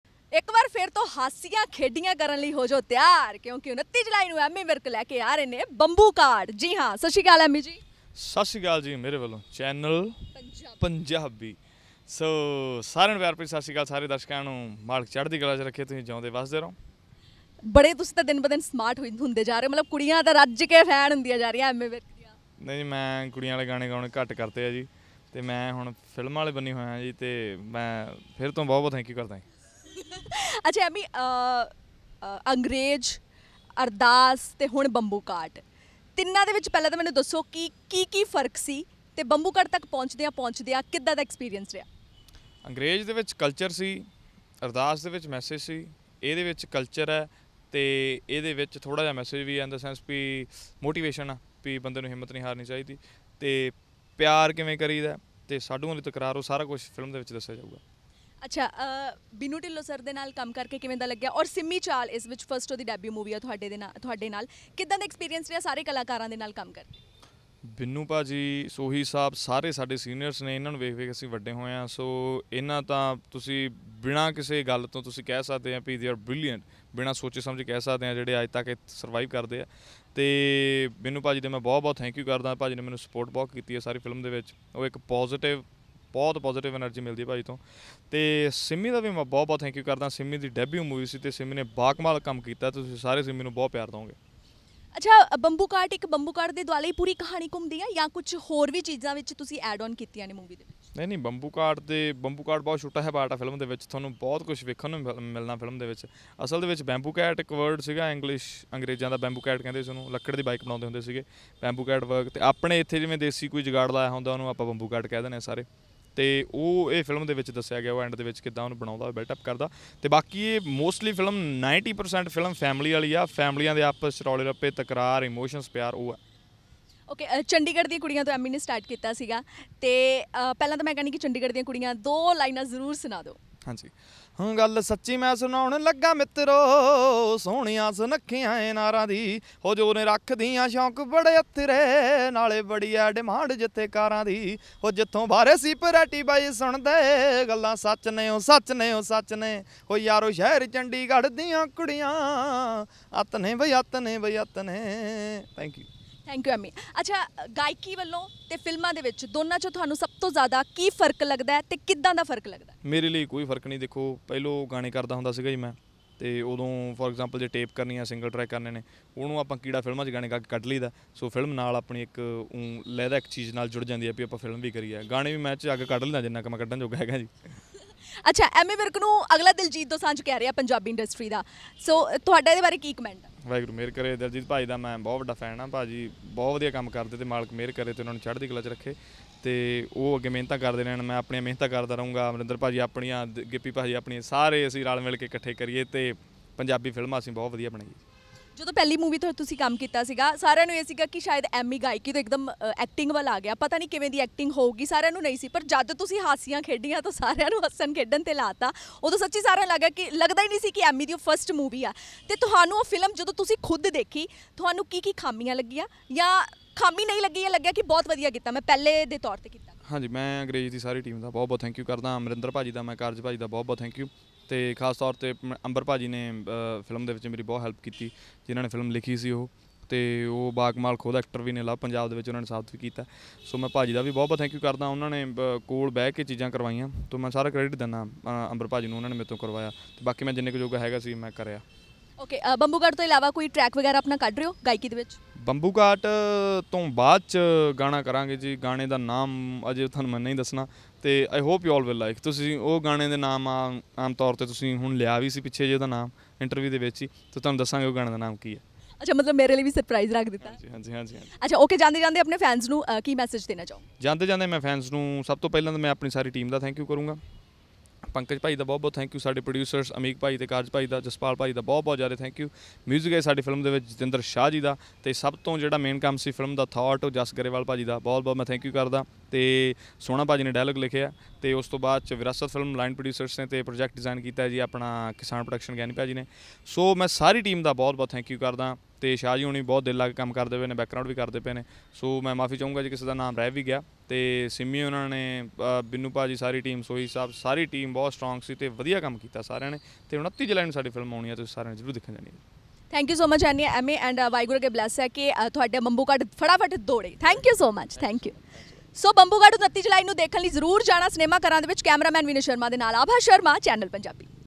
Ammy Virk | Bambukat | Exclusive Interview | Channel Punjabi
Host: Abha Sharma
Actor & Singer : Ammy Virk
Cameraman: Vinay Kumar
Edited by: Sadhvi Jethi